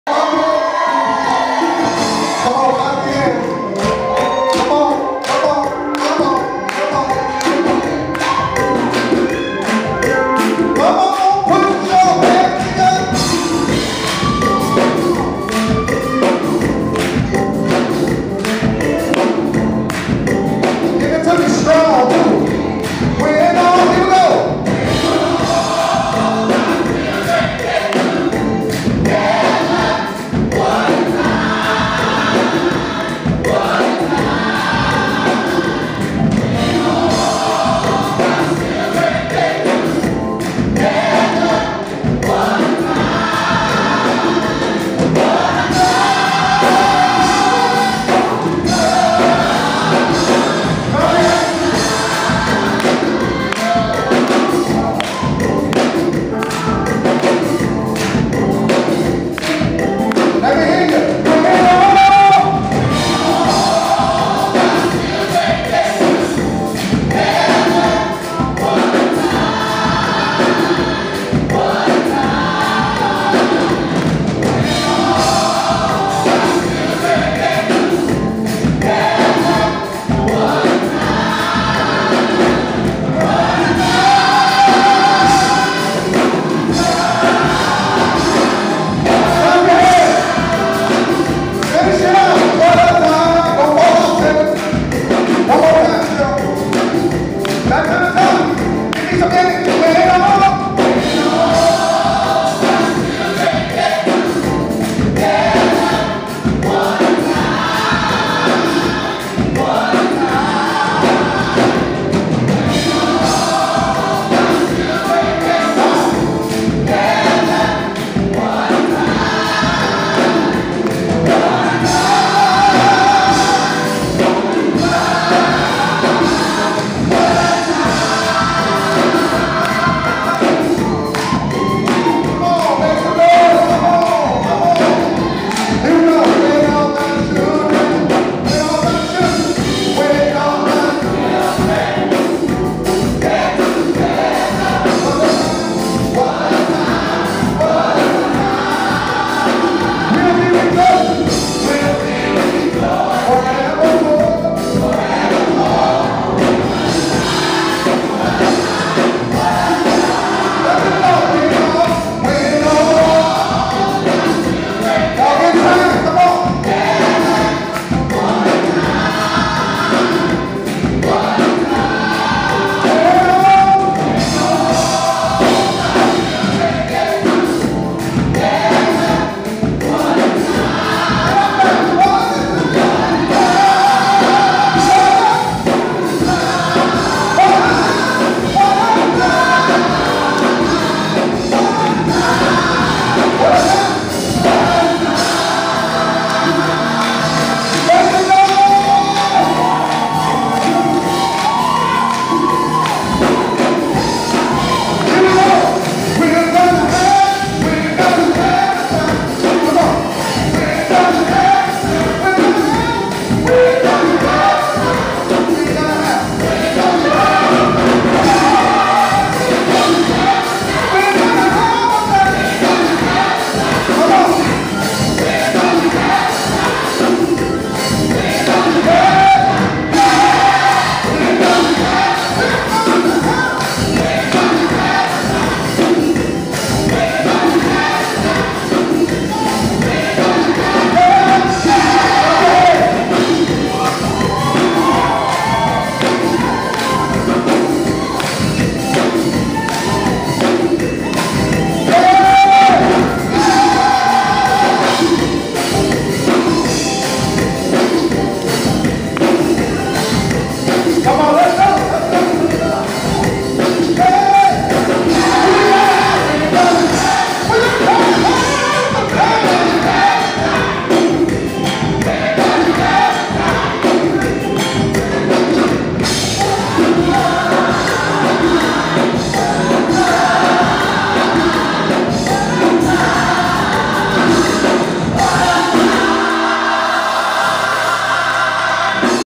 The Concert
This Choir will be gathering as a whole one more time to celebrate life, legacy and living for God. (4-24-16). Here is a glimpse into what it will sound like as they prepare.